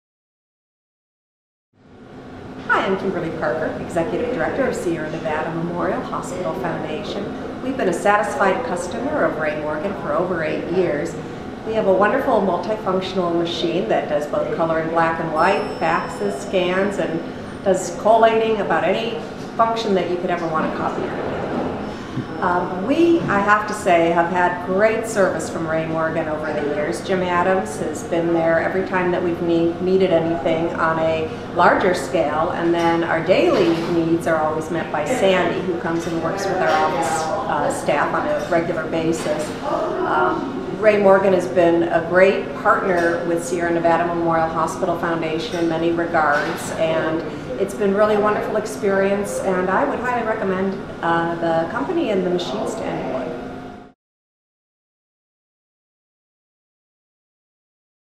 Ray Morgan Company & Sierra Nevada Memorial Hospital: A Testimonial - Nevada City, CA
Kimberly Parker, Executive Director at Sierra Nevada Memorial Hospital Foundation, explains how RMC has been a great partner and supporter of SNMH.